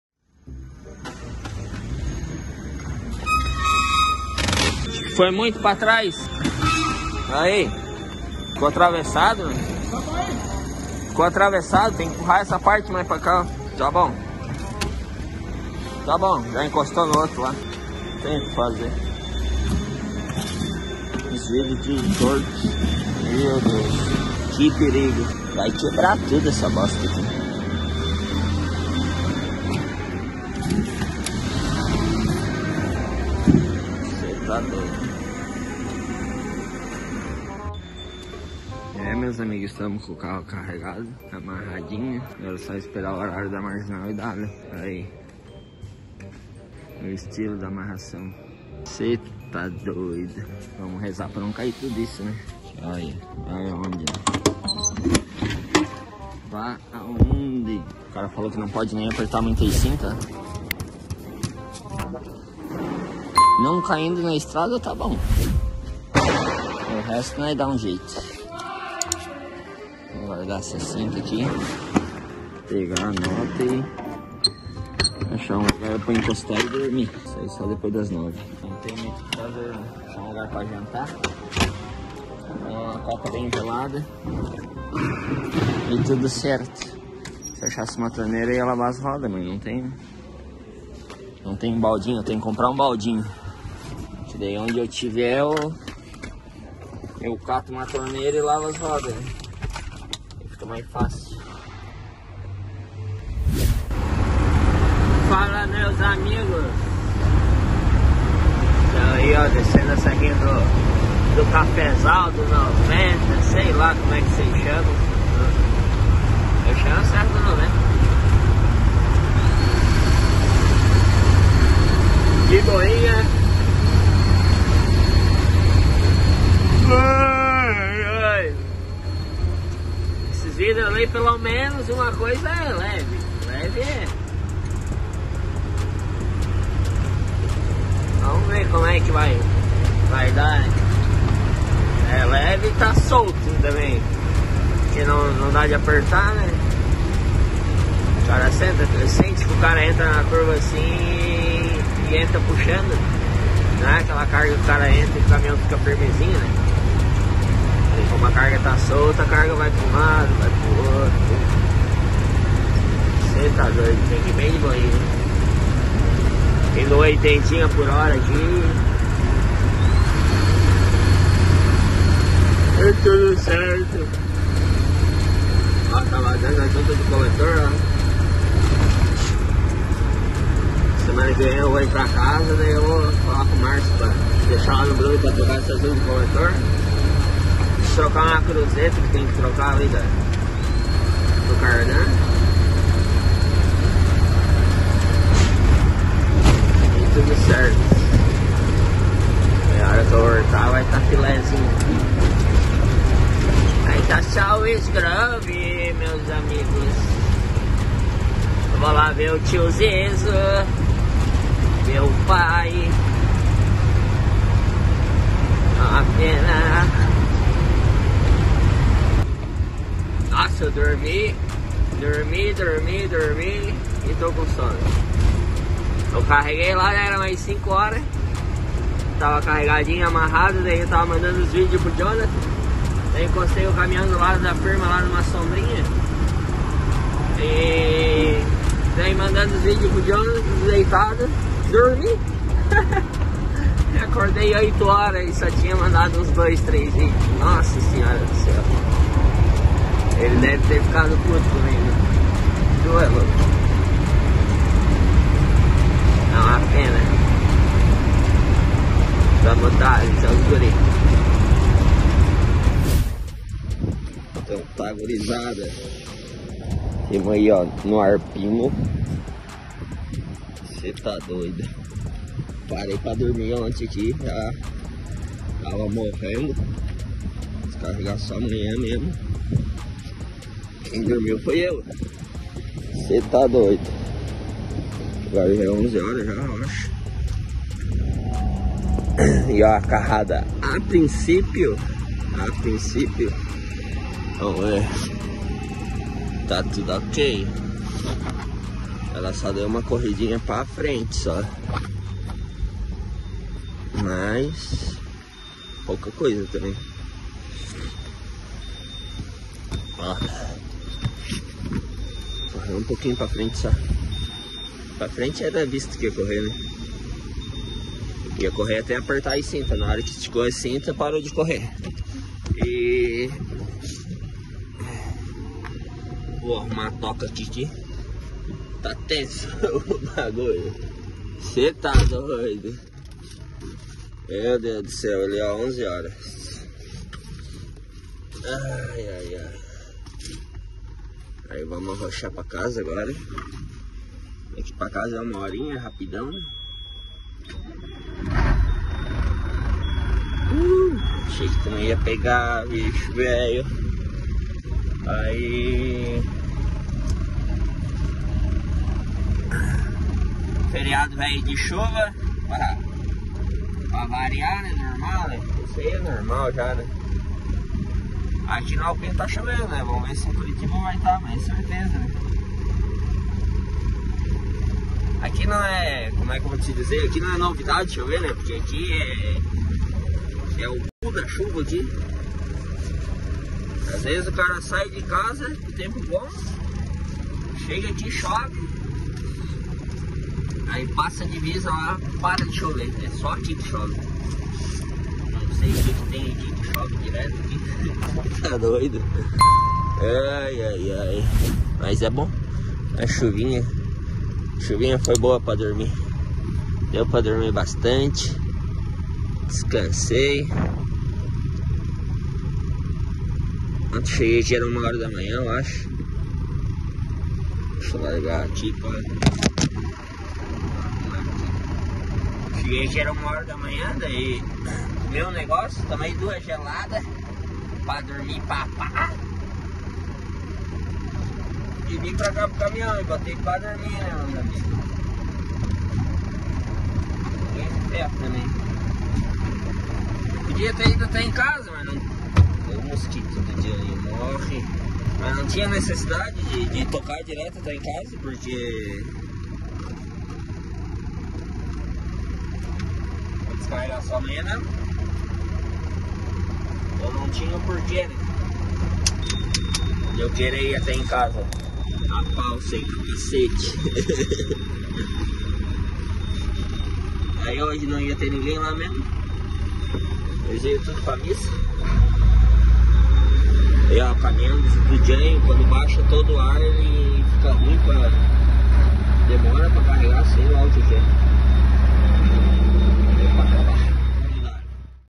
NÃO DEIXARAM EU AMARRAR A CARGA! OLHA O PORQUE!
00:00-Finalizando o Carregamento!
00:37-Não Deixaram eu amarrar!
01:50-Descendo a serra do 90!
04:48-Olha como ficou a carga!